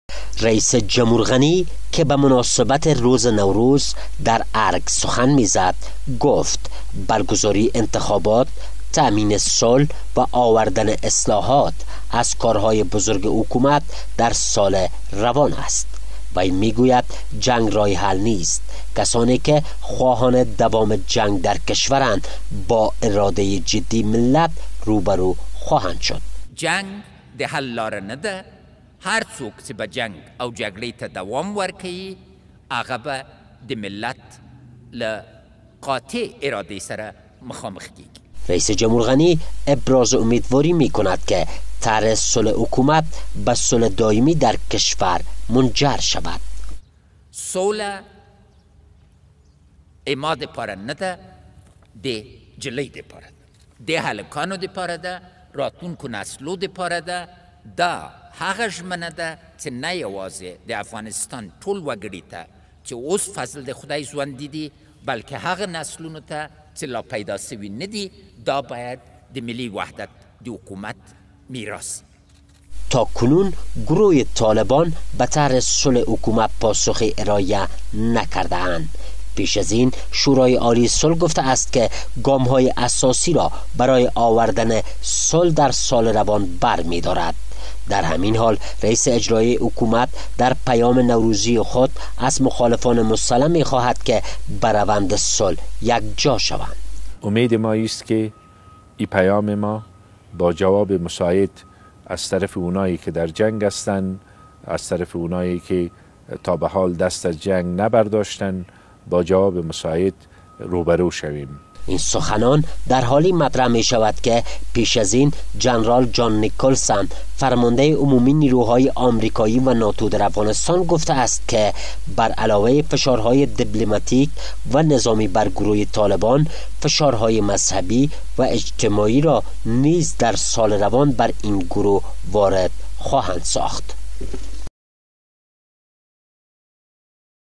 سال پیشرو سال پرکار برای حکومت است /گزارشی از حفیظ الله امین سالنگی 01 01 1397
رییس جمهور غنی در پیام به مناسبت روز نو روز گفت سال پیشرو سال پرکار در بخش ها اصلاحات اداری به ویژه در پولیس برگزاری انتخابات وتلاش ها برای  آمدن صلح در کشور خواهد بود .